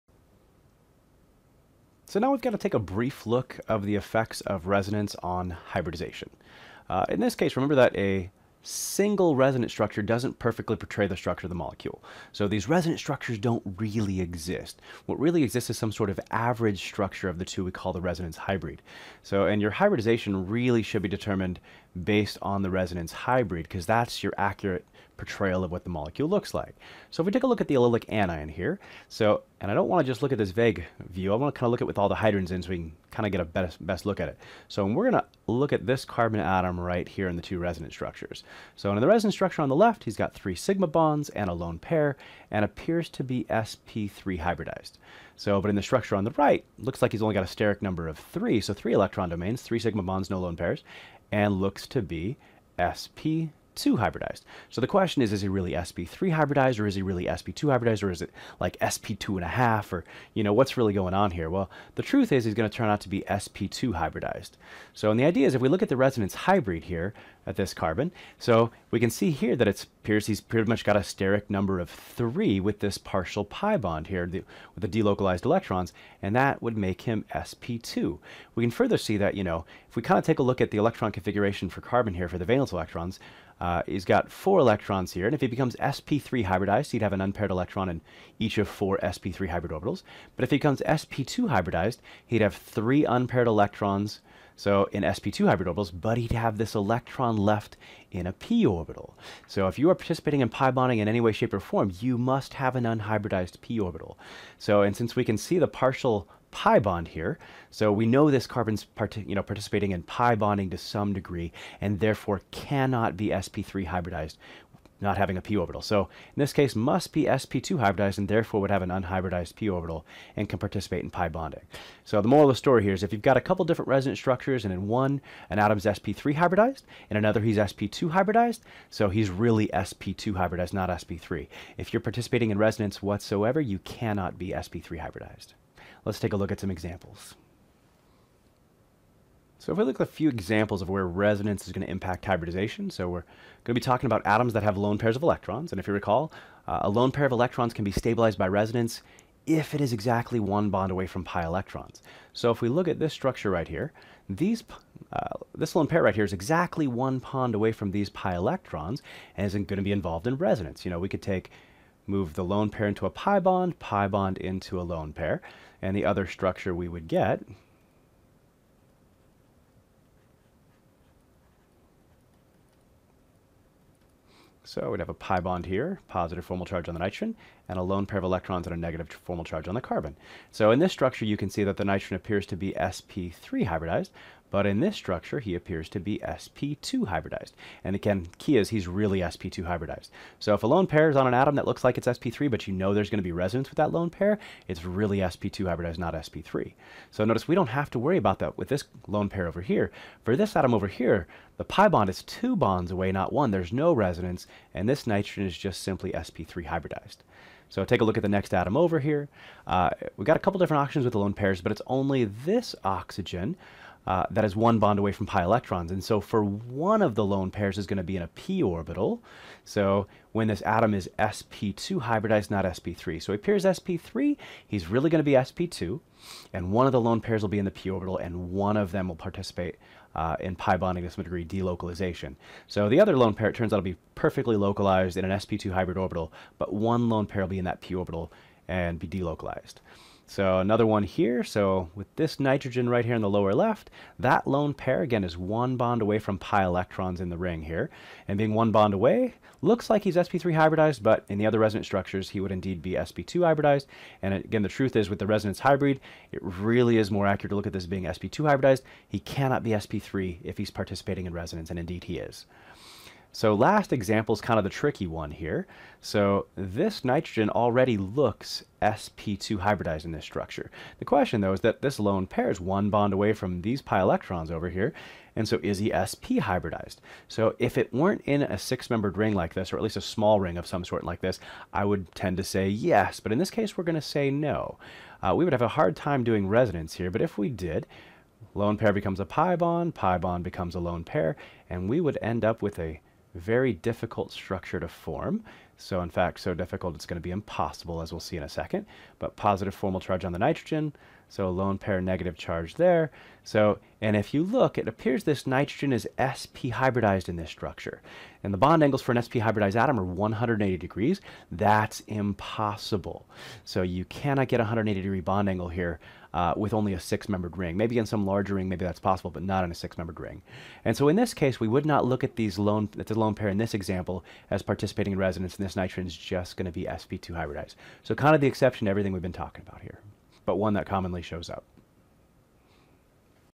2.3b Resonance and Hybridization
Chad breaks down how the resonance hybrid must be considered in determining the hybridization of certain atoms in resonance structures.

0:00 - Effects of Resonance on Hybridization
2:19 - Resonance and Hybridization Examples

Happy Studying!